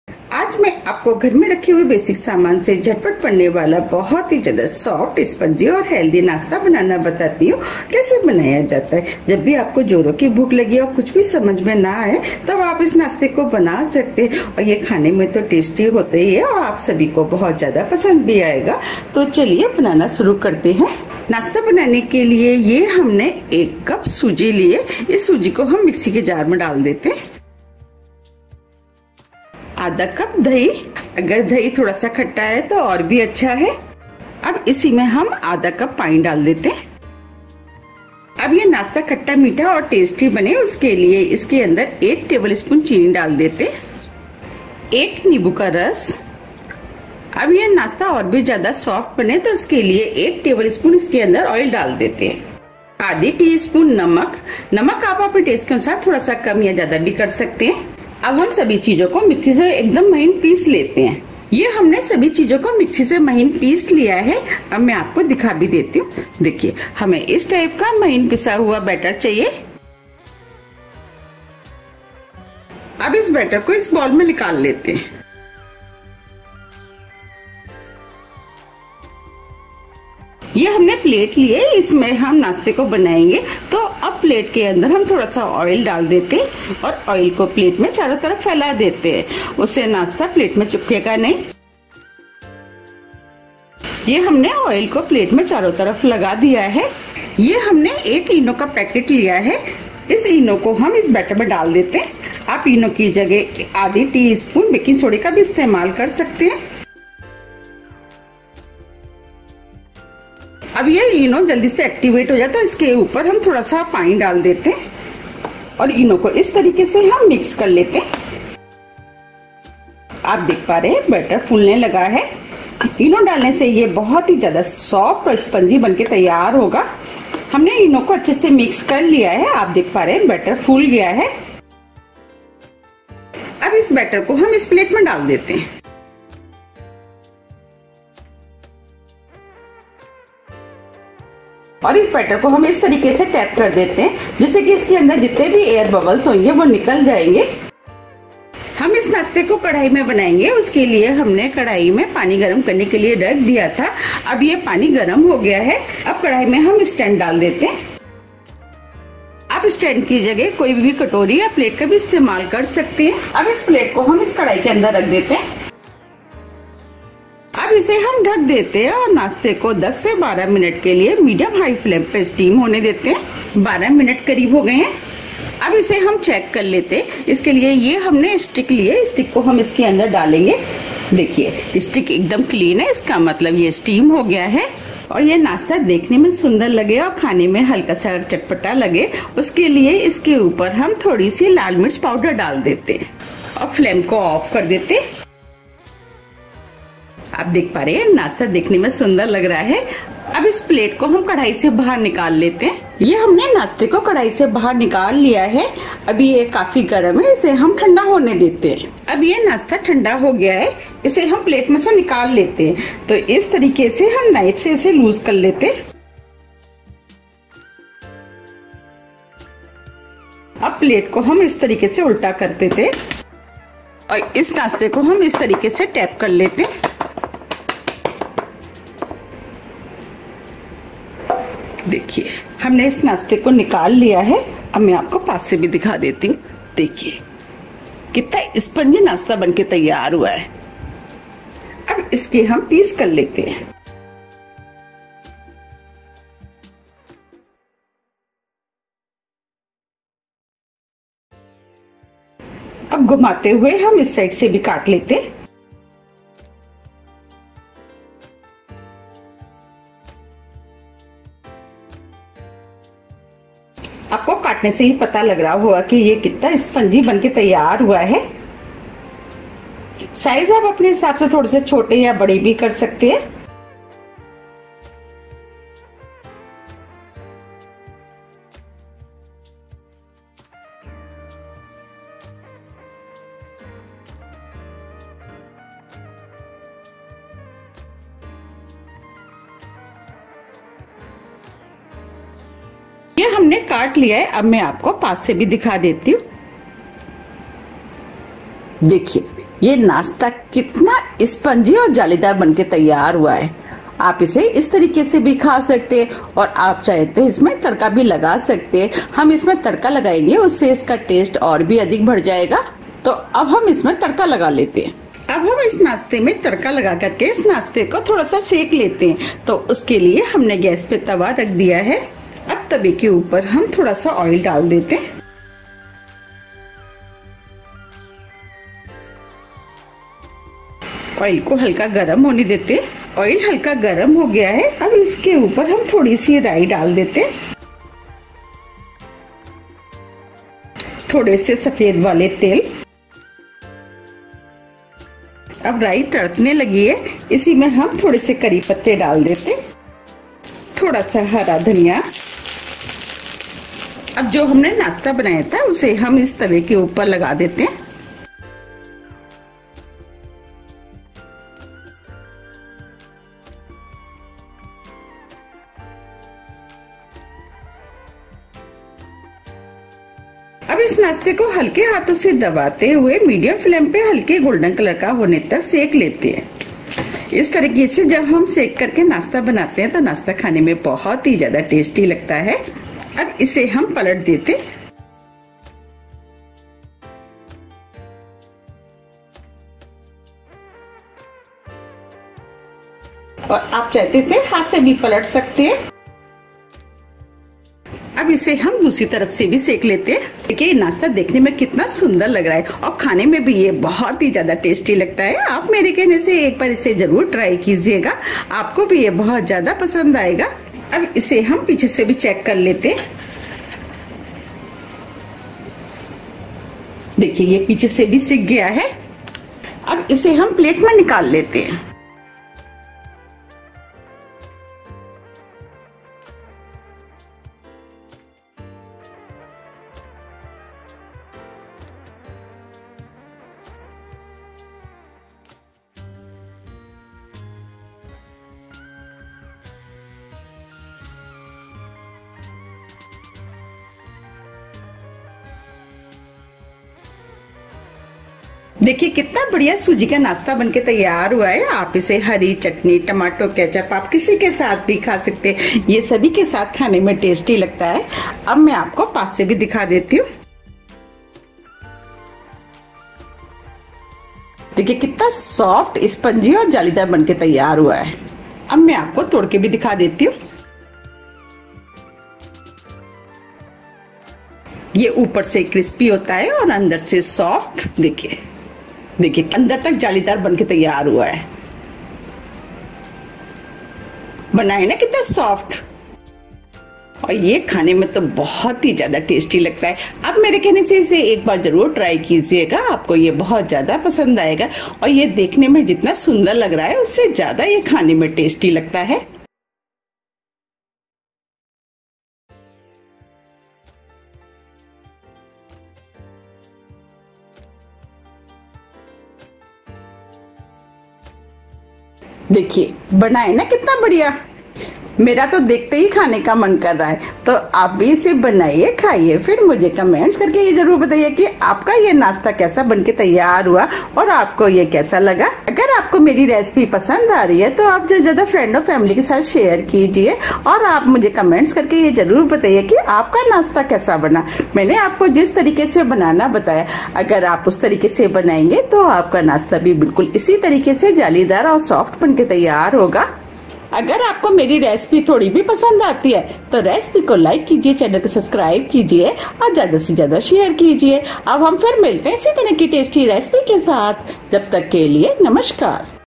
तेज भूख मे सूजी से बनाए पूरे परिवार के लिए एकदम हल्का फूल्का और टेस्टी नास्ता वो भी आसानी से Nashta
Snacks | Semolina Snacks Recipe

Ingredients Required-
1 cup semolina
1/2 cup curd
Water
1 tbsp sugar
1 squeeze lemon juice
1 tbsp oil
1/2 tsp salt
1 sachet ENO
Red chilli powder
Oil
Mustard seeds
Sesame seeds
Curry leaves
Coriander leaves